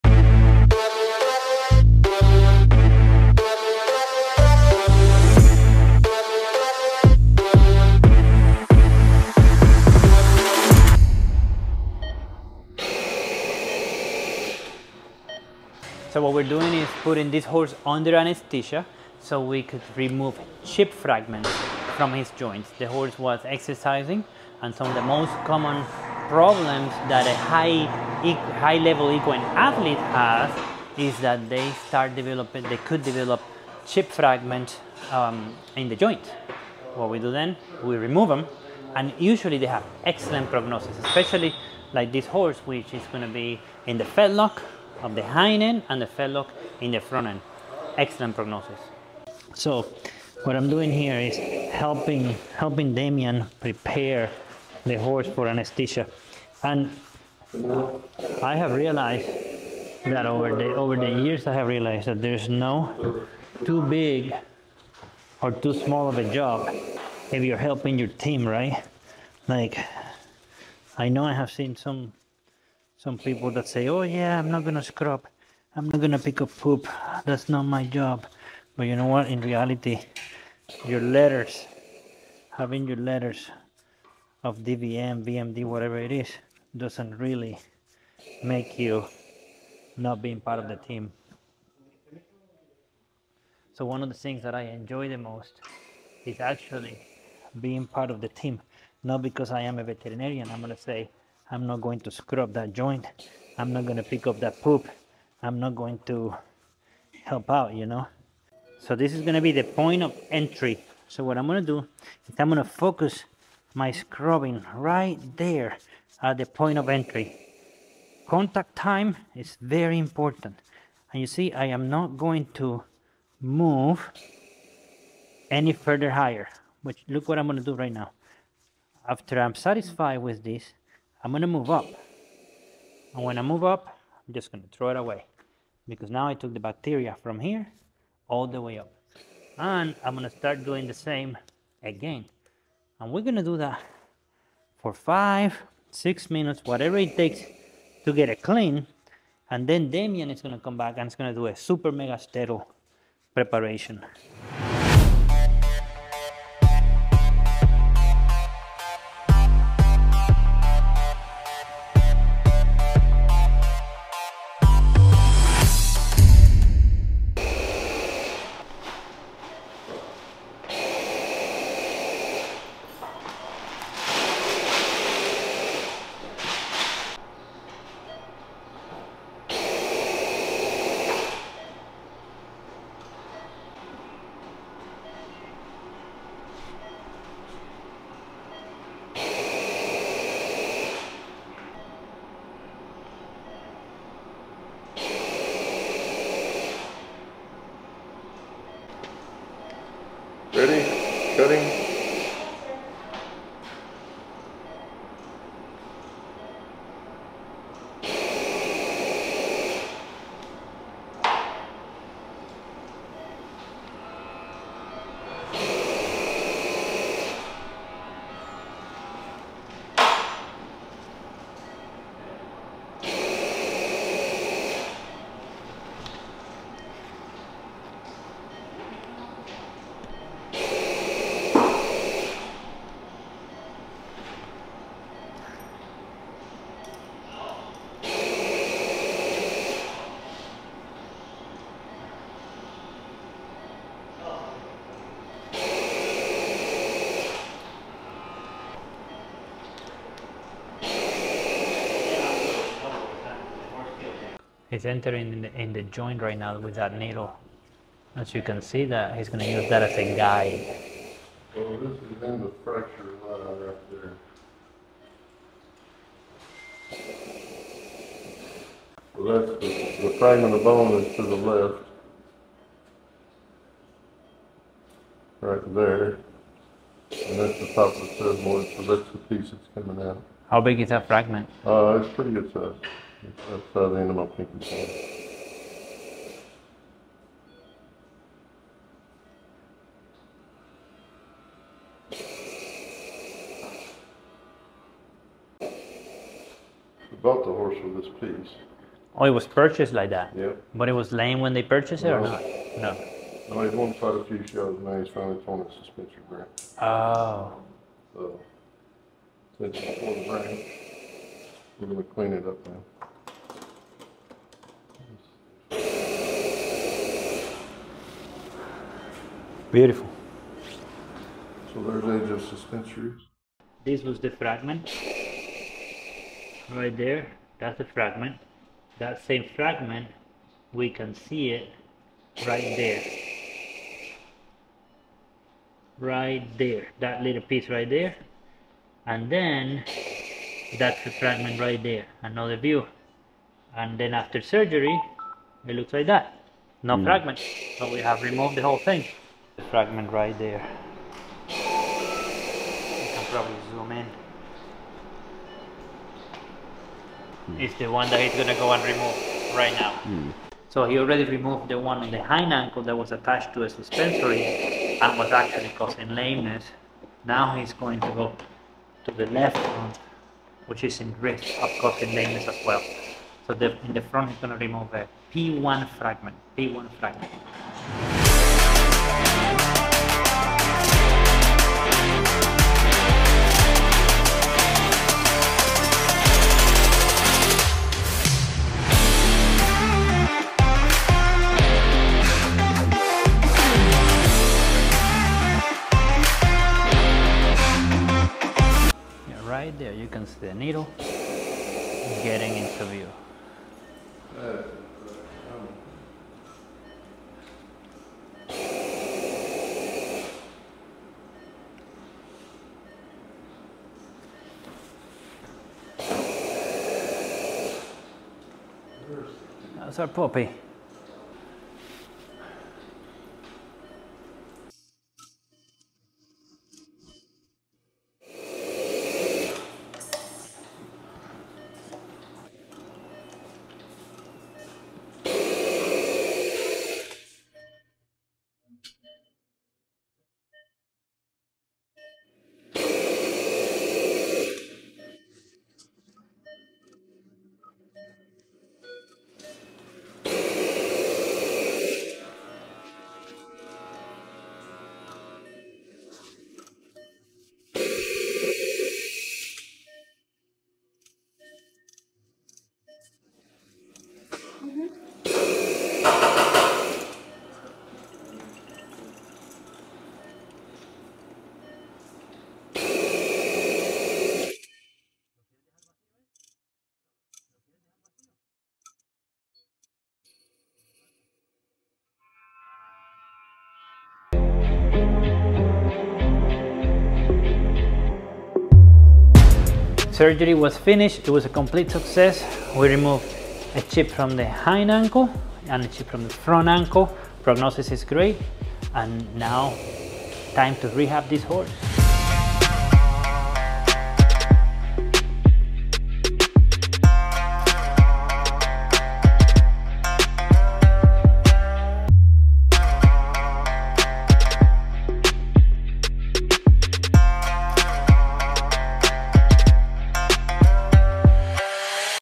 Arthroscopic Removal of Fetlock Joint Fragments in a Performance Horse
Arthroscopic removal of fetlock joint fragments is a critical procedure for high-performance horses. In this video, we showcase the step-by-step process of removing osteochondral fragments from the fetlock joints of an equine athlete. These fragments, commonly caused by intense exercise, can lead to lameness if left untreated.

Watch as our expert veterinary team administers anesthesia, prepares the surgical site, and performs arthroscopic removal of fetlock joint fragments with precision. The procedure ensures a clean joint, reduces future risks, and improves the horse's overall prognosis.

If you're looking to understand more about arthroscopic removal of fetlock joint fragments and its role in equine rehabilitation, this video provides a detailed overview of the process and outcomes. Join us as we take this horse through a successful surgery and set it on the path to recovery.